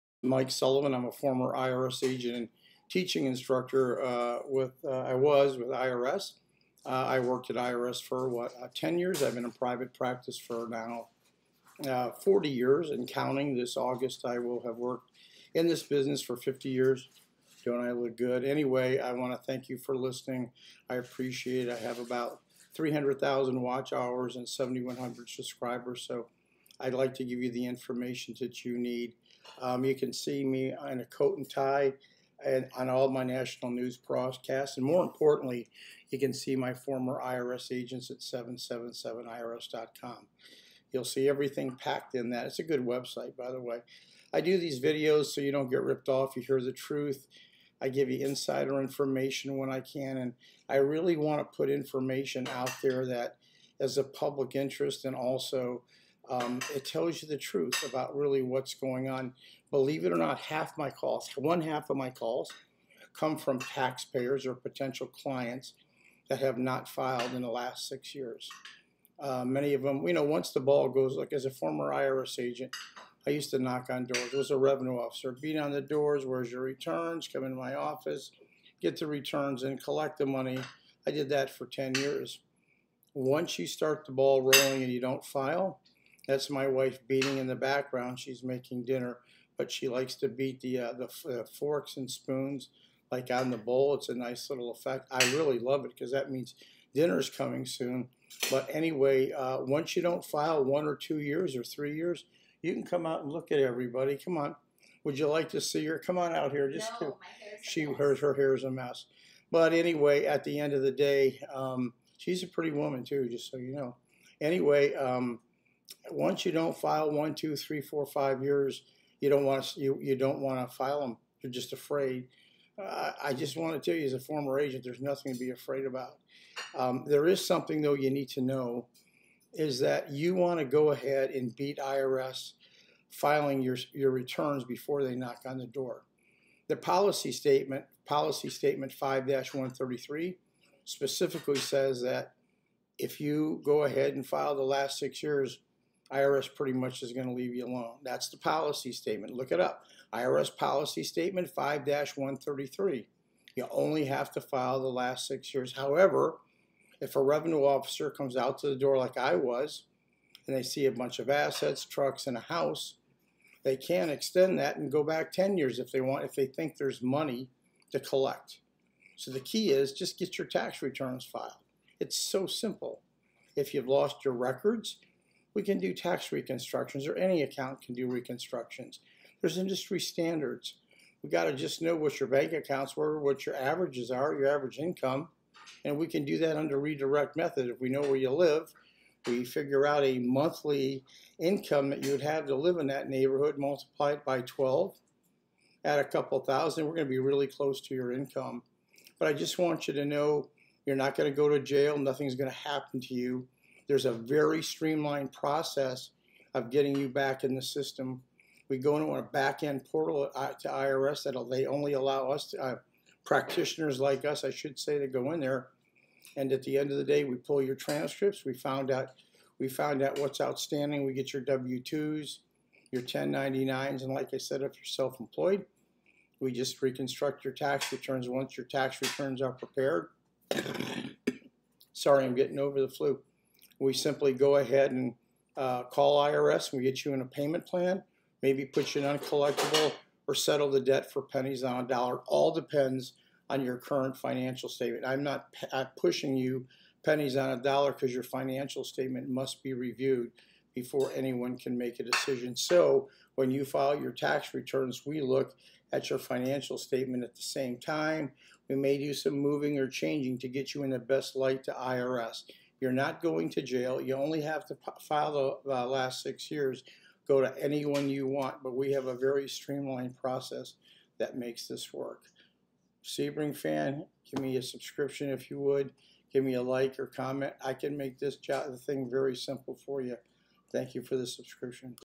Former Agent Knows The System, Unfiled Back Tax Returns, EZ Streamline Process/System Worry Free,
former IRS agent and teaching instructor Mr. Sullivan, a former revenue officer can get you back in the system worry free.

Get your tax returns filed and work out a payment plan, currently not collectible, or file an offer in compromise.

We are available for free initial tax consultation..